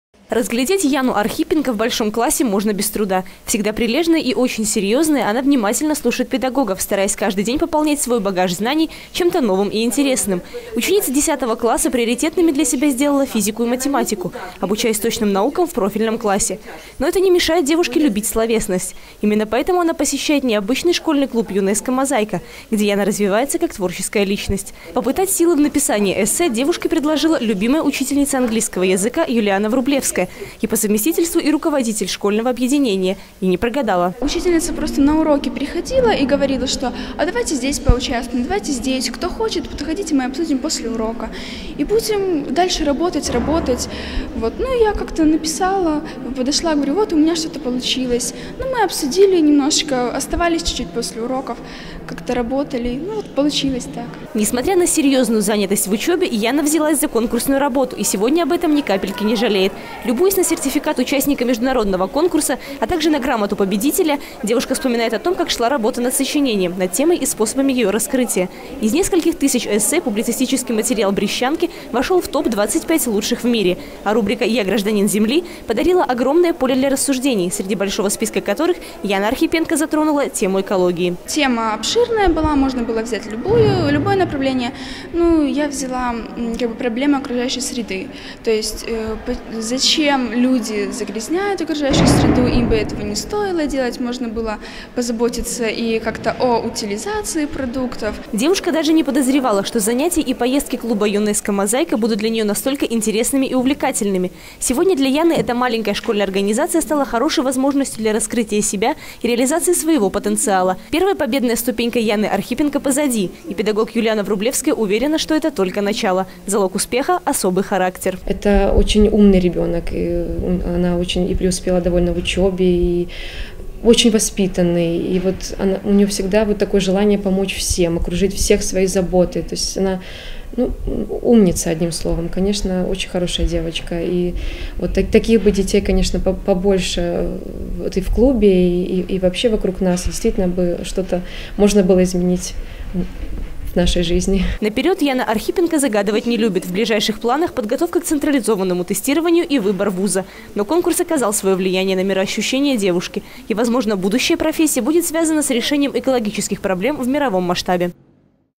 2015-01-27 г. Брест.Портретная зарисовка: Яна Архипенко. Телекомпания Буг-ТВ.
Портретная зарисовка: Яна Архипенко – победительница Международного конкурса эссе.
На протяжении многих лет берестейские школьники уверенно держат высокую планку во многих сферах - образовании, культуре, спорте. Они неоднократно являлись и являются победителями олимпиад, конкурсов, спортивных состязаний разного уровня, прославляя регион далеко за его пределами.
Героиня следующего сюжета выпуска Яна АрхИпенко - победительница международного конкурса эссе, объявленного всемирной организацией ЮНЕСКО в 2014 году. Чтобы познакомиться с талантливой девушкой наша съёмочная группа отправилась в гимназию №1, где узнала много нового о её взглядах и интересах.